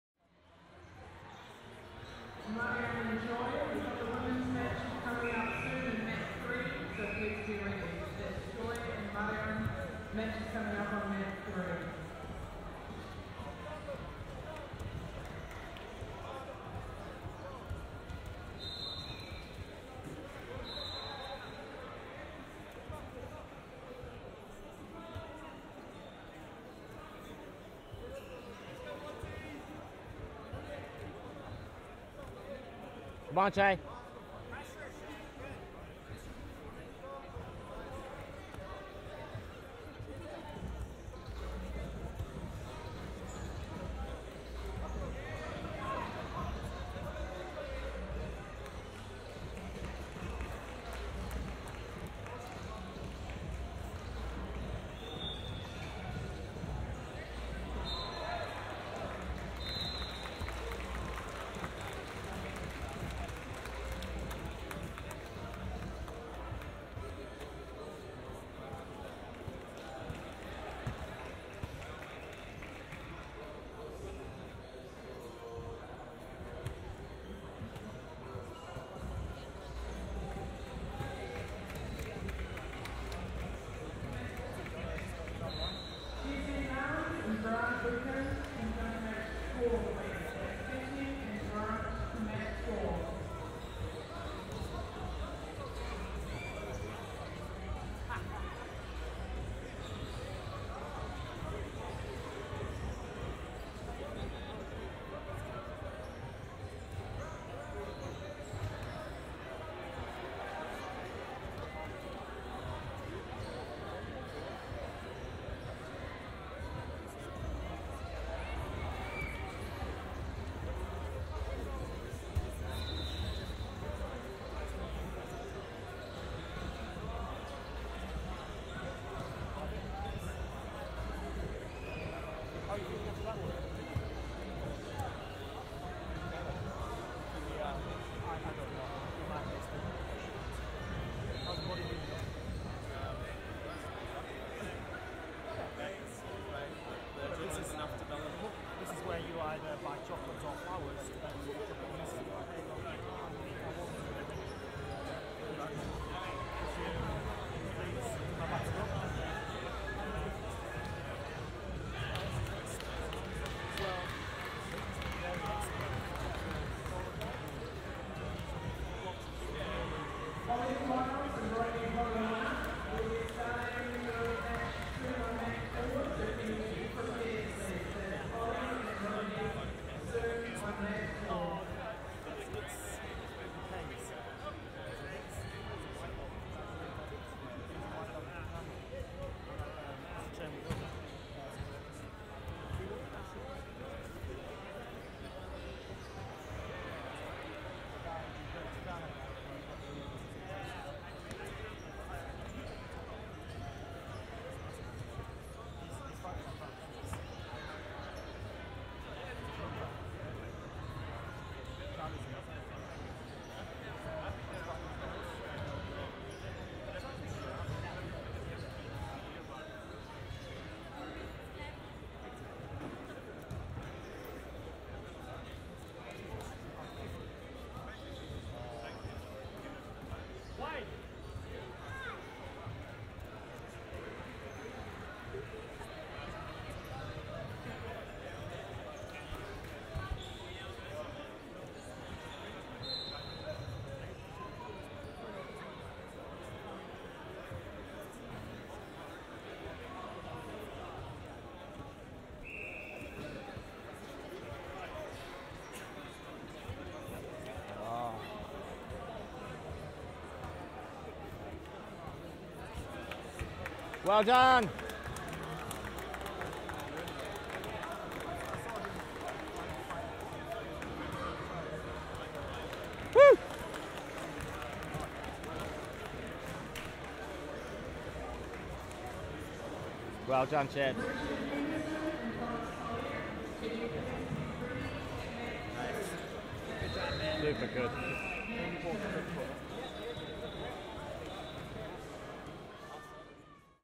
Chay Johansson and Ahamed Sithik - Wellington BJJ No-Gi Open Tournament
Chay Johansson [GSW] and Ahamed Sithik [GSW] face off in the first Men's Beginner round [Men's 91.5kg] of the Wellington BJJ No-Gi Subs-only Open Tournament on April 13th 2019 in Kilbirnie, Wellington at the ASB Sports Arena.

Soundtrack features binaural audio - put your headphones on and turn it up to feel like you're really beside the mat.